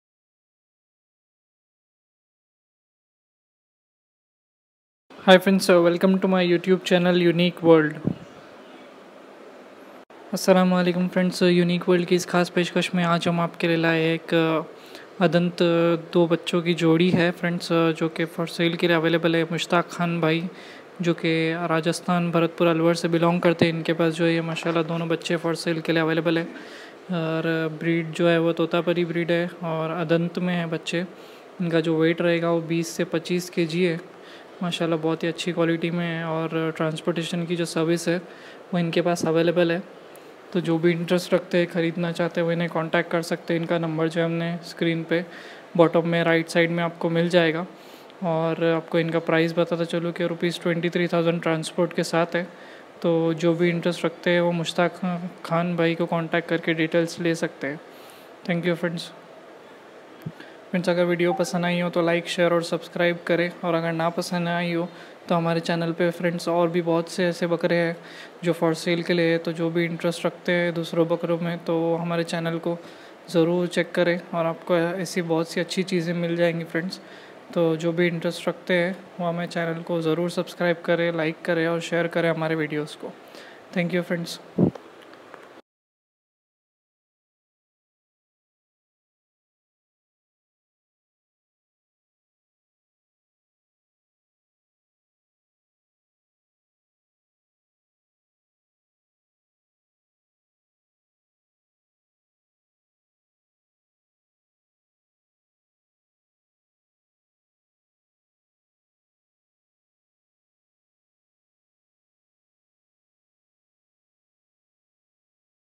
Totapari Kids Jodi For Sale Contact Mushtak Khan
Age : Adant
Weight : 20-25kg
Location : Alwar Rajasthan

Transportation Available All over India